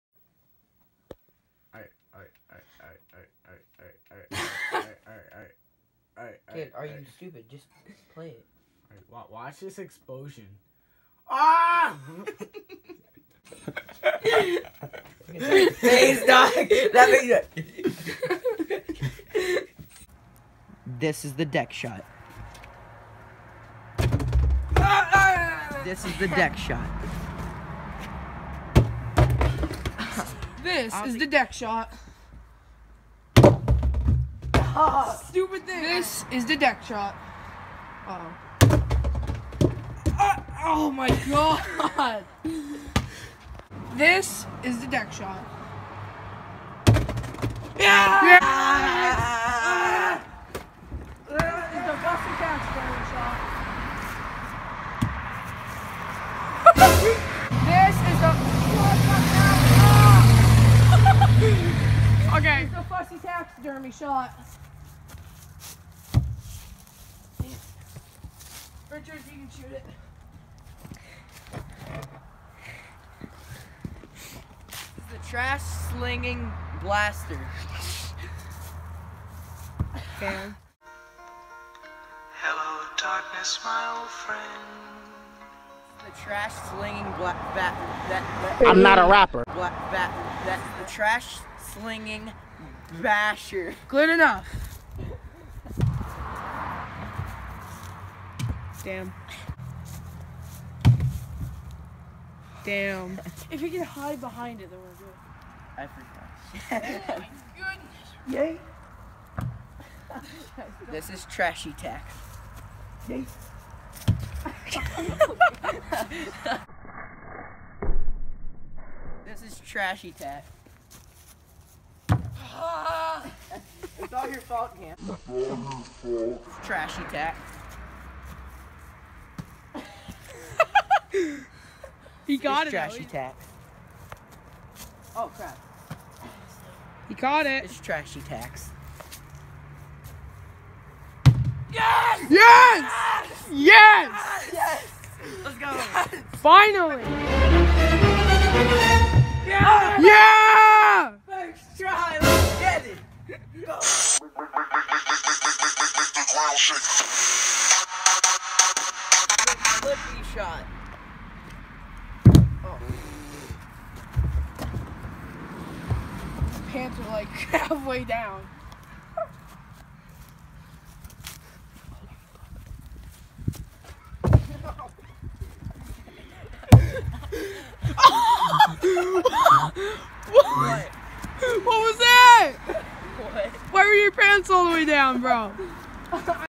Funny Bloopers from Dud Perfect Trickshot video
Ik this happened about a month and a half ago but I found the old Bloopers and decided to make a Bloopers video to it. It's actually pretty funny.